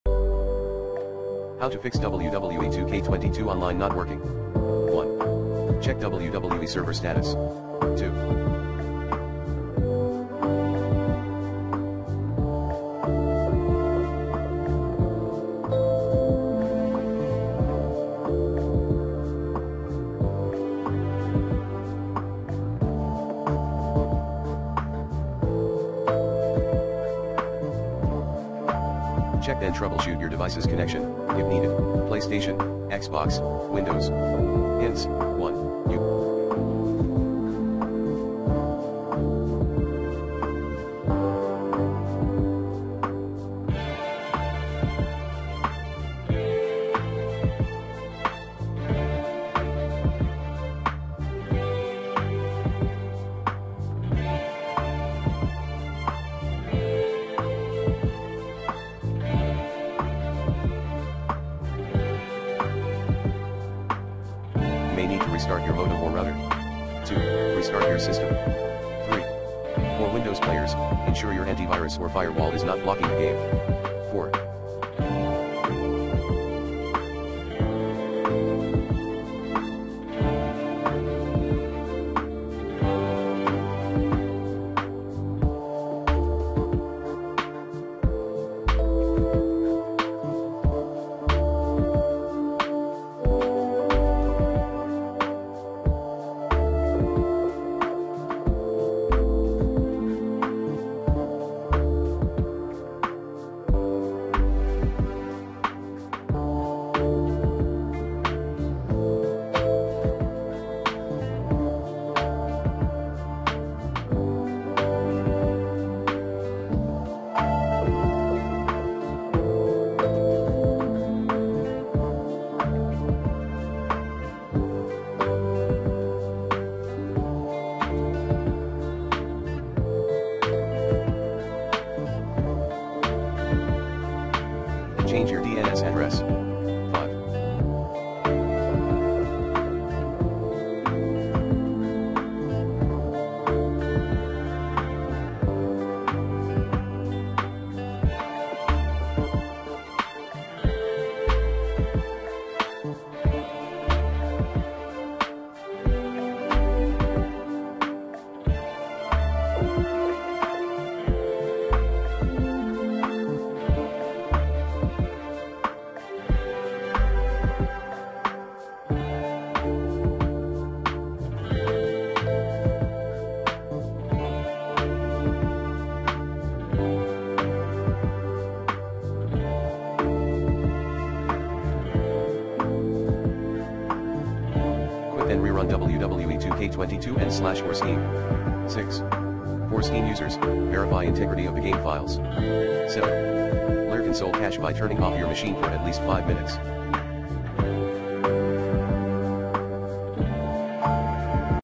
Fix WWE 2K22 Online Not Working issue on PS5/PS4/Xbox/Windows
Here's how to Fix WWE 2K22 Online Not Working issue on PS5/PS4/Xbox/Windows.